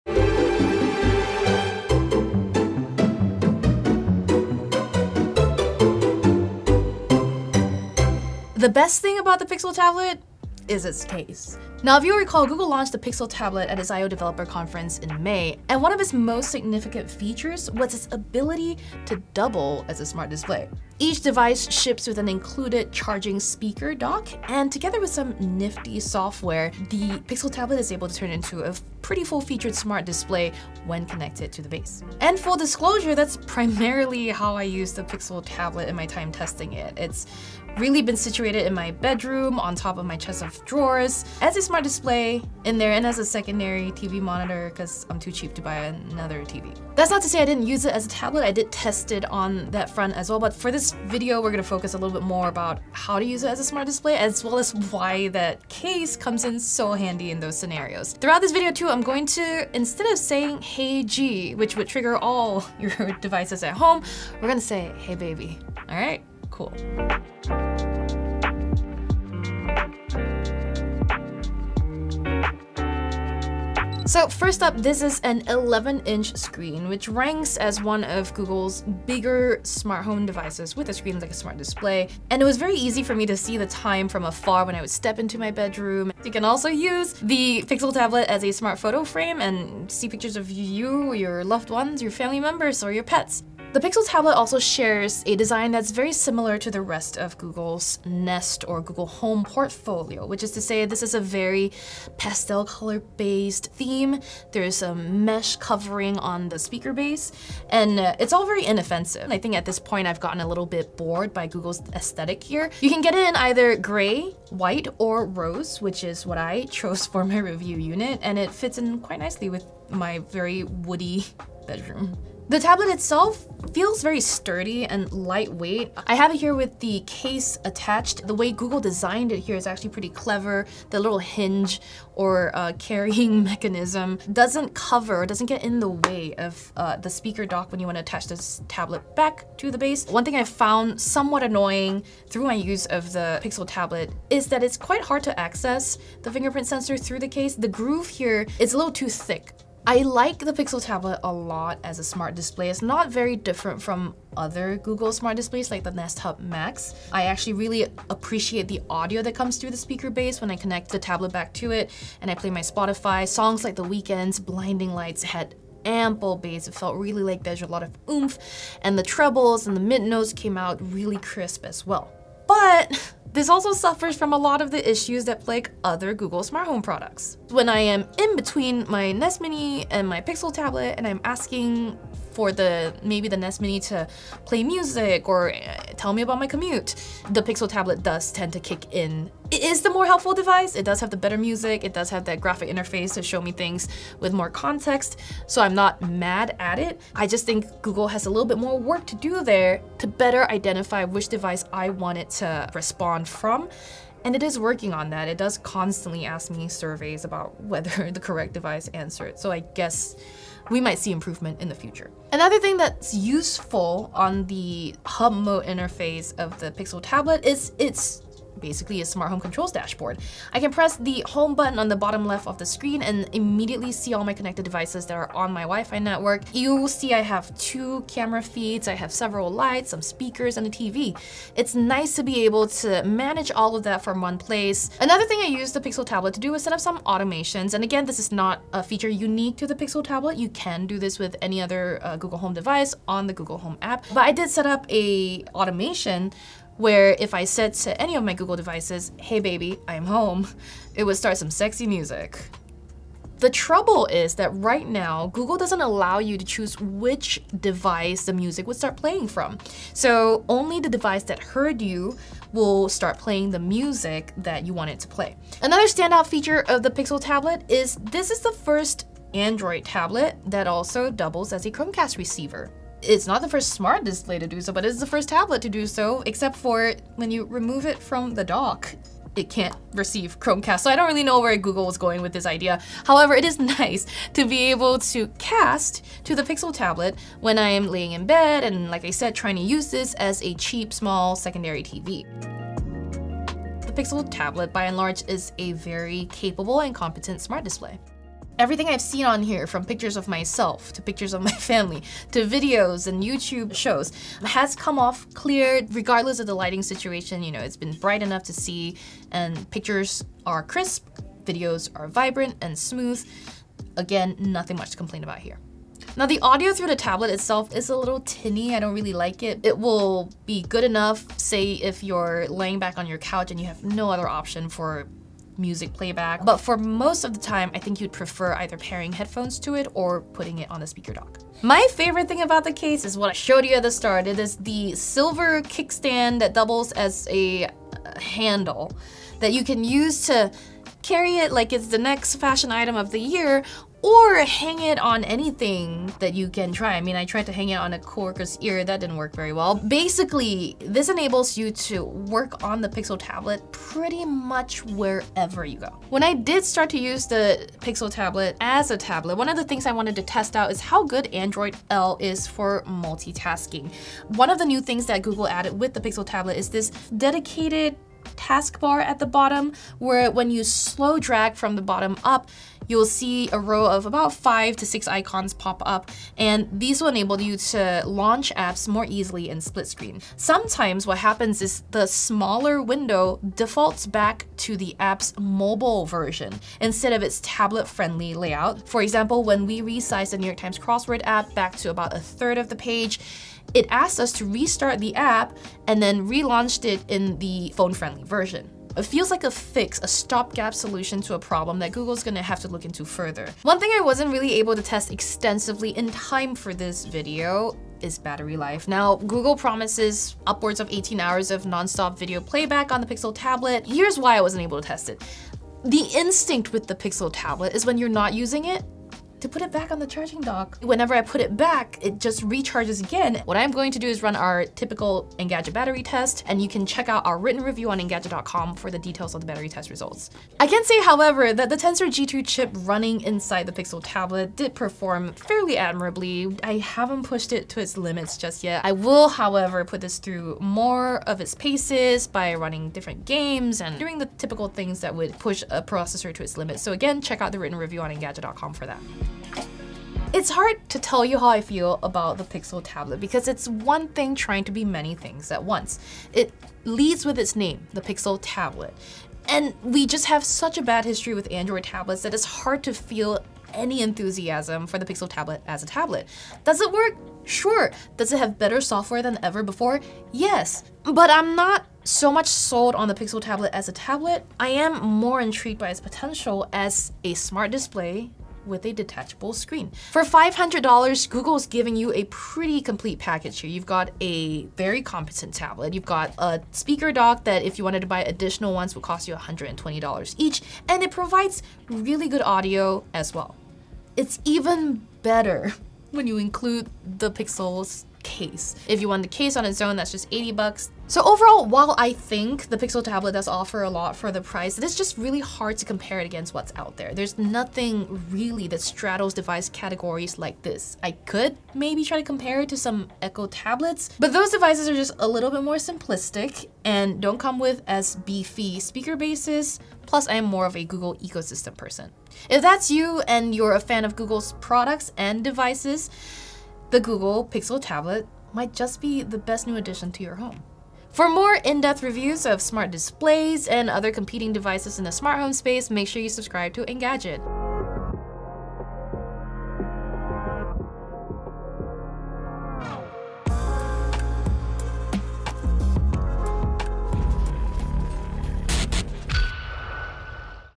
Google Pixel Tablet review: Clever accessories transform an unexciting tablet
The Pixel Tablet is a competent smart display that is also a tablet always charged and ready when you need it. Some quirks aside, it's a versatile product that deserves a spot in your home.